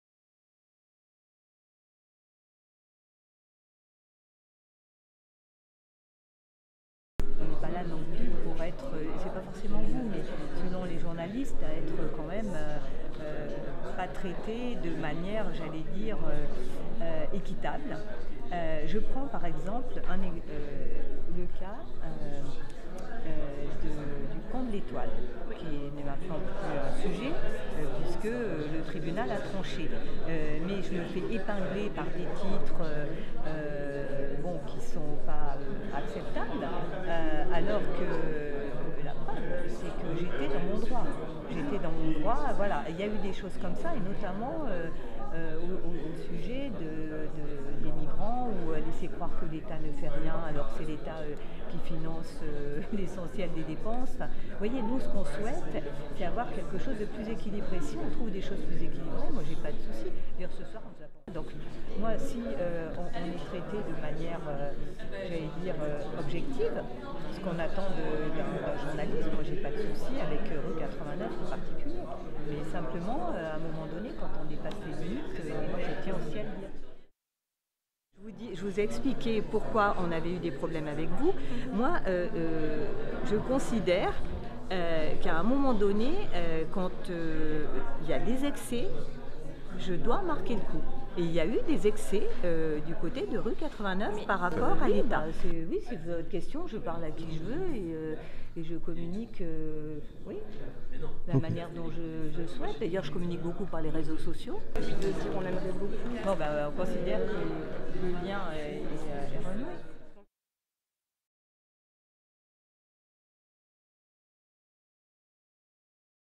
La préfète Josiane Chevalier s'explique sur ses relations avec Rue89 Strasbourg
Après plus d'un an de boycott de Rue89 Strasbourg, la préfète du Bas-Rhin Josiane Chevalier détaille ses griefs par rapport au travail de Rue89 Strasbourg et amorce une reprise d'un travail normal avec notre média.